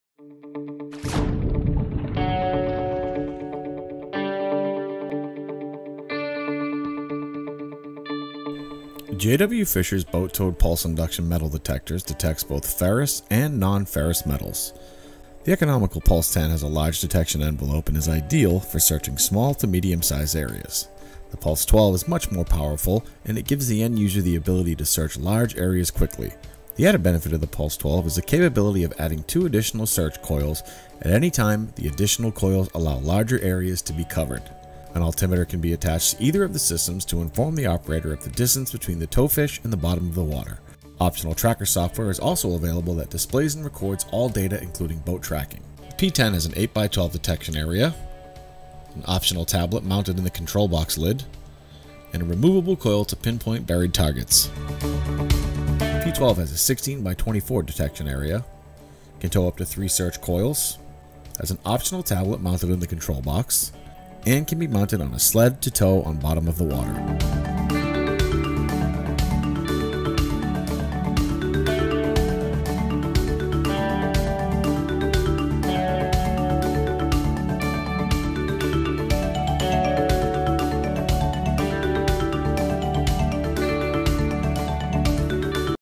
JWFishers Pulse 10 & Pulse 12 Boat towed metal detectors
Fishers boat-towed pulse induction metal detectors detects both ferrous (iron/steel) and nonferrous (gold/silver/aluminum/bronze/etc.) metals.